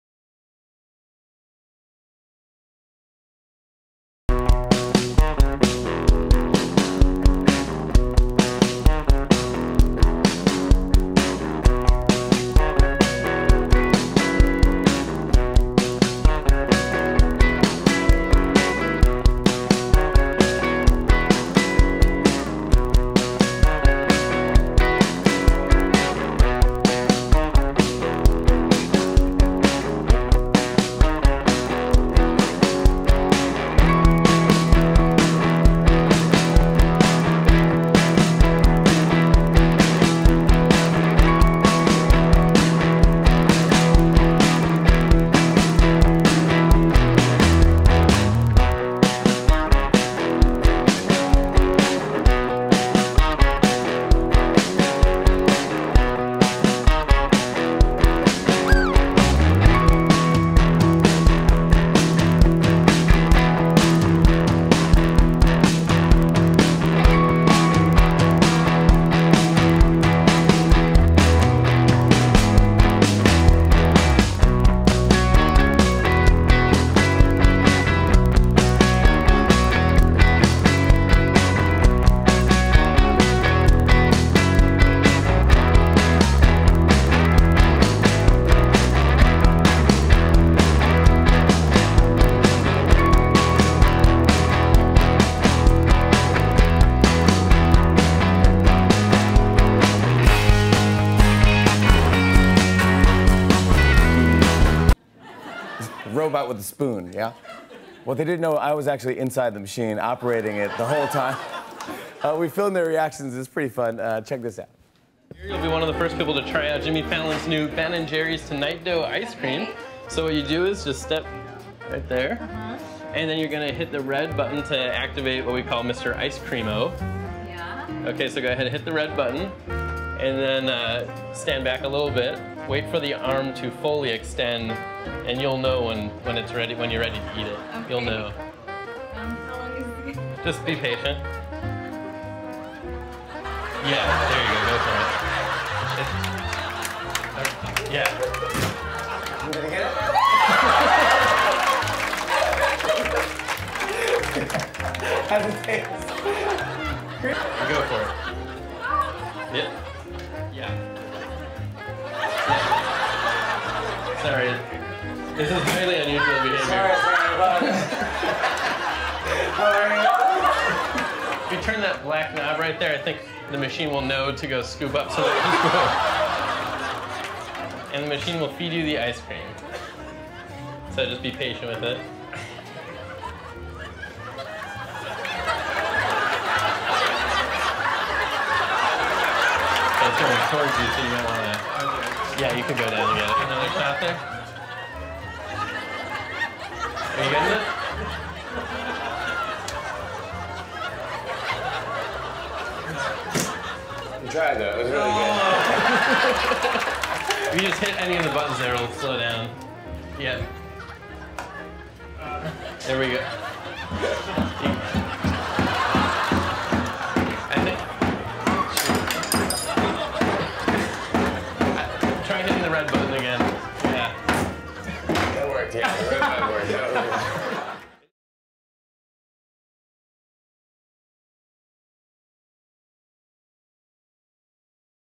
The Tonight Dough by Underground Visuals [HD]
Production of an interactive sign built for The Tonight Show with Jimmy Fallon by Underground Visuals